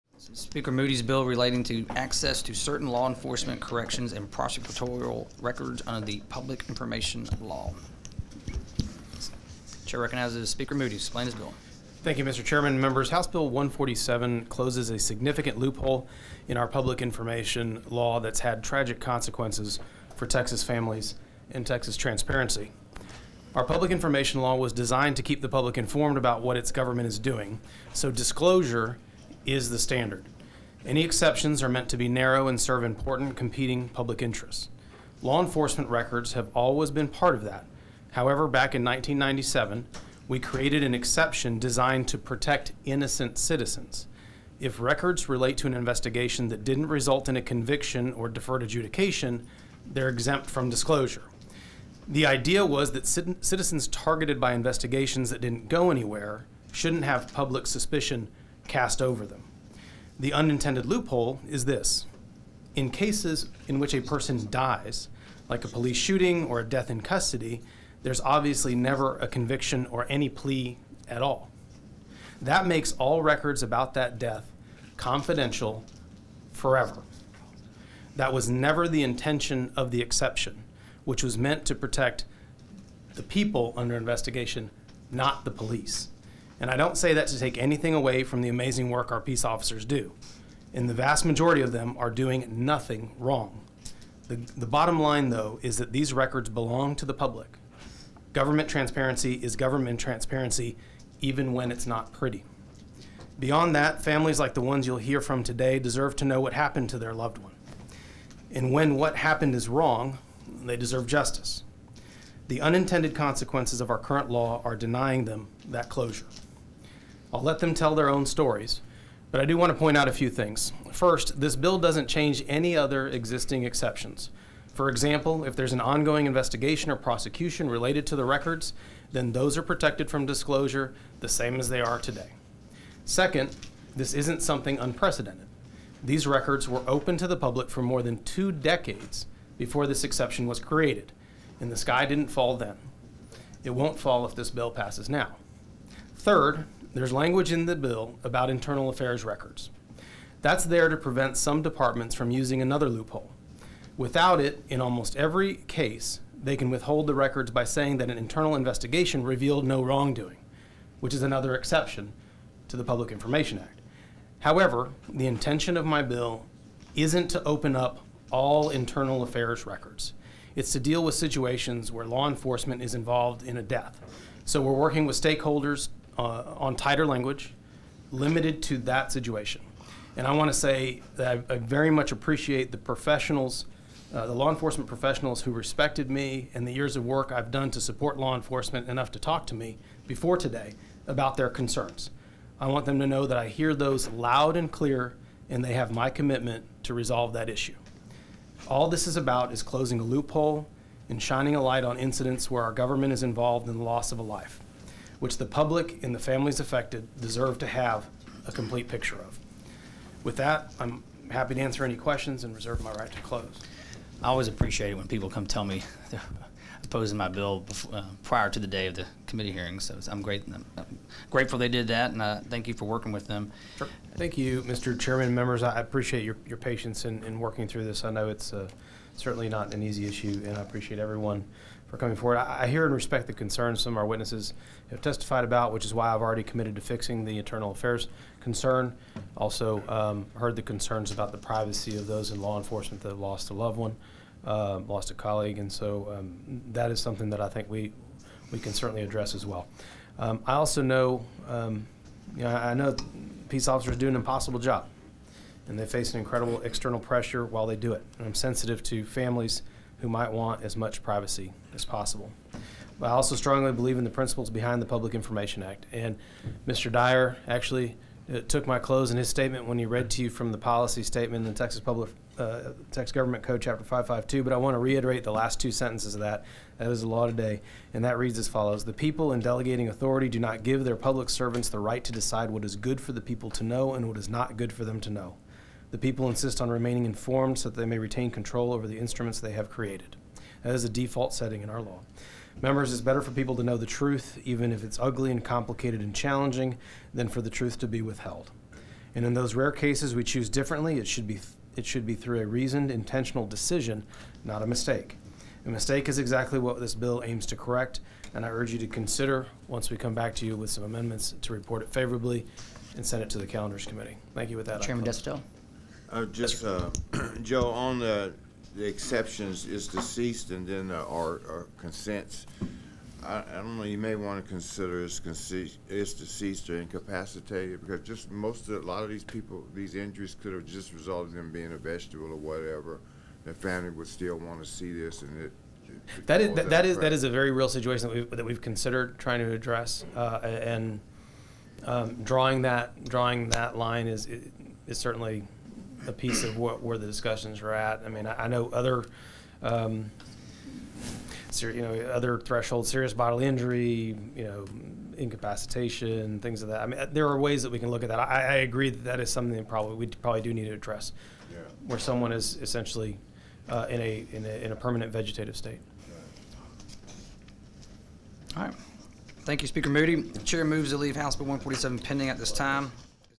Rep. Joe Moody, D-El Paso, talks about HB 147
Rep. Joe Moody, D-El Paso, authored legislation to require police to release information in closed cases if a suspect is dead or agrees to its release.